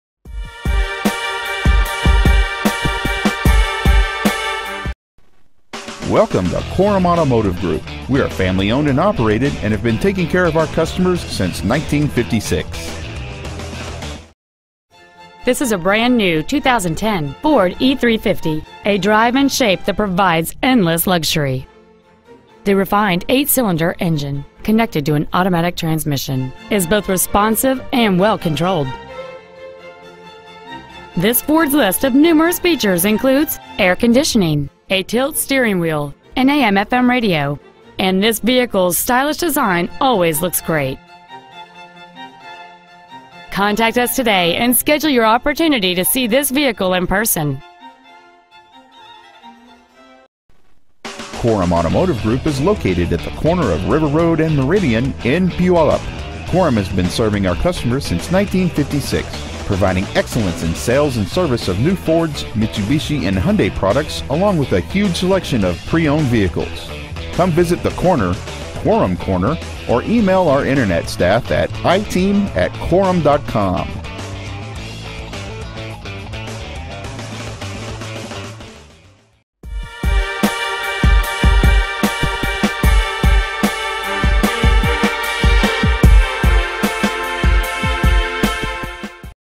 2010 Ford E-350 Puyallup WA
We are proud to present this 2010 Ford E-350.

 Year: 2010
 Make: Ford
 Model: E-350
 Engine: 5.4L V8
 Trans.: Automatic
 Exterior: White
 Miles: 25
 Interior: Flint

 2010 Ford E-350 Puyallup WA